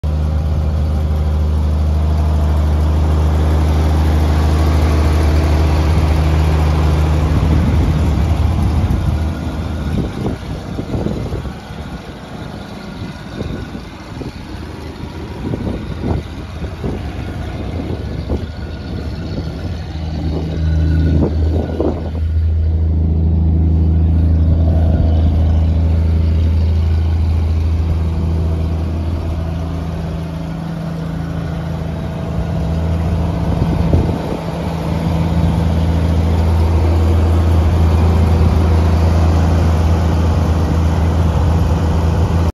1953 IH R-162 Dump Truck Video1
1953 IH R-162 Dump Truck, gas engine, 93HP, Eaton 4 speed transmission, hydraulic bed lift, steel bed 8 foot long by 7ft wide, chained lift gate, 3 passenger cab, manual locks and windows, spring ride suspension, single axle duals, 8.25-20 tires, 63,484 miles.
DPA Auctions
10/17/23 Online Auction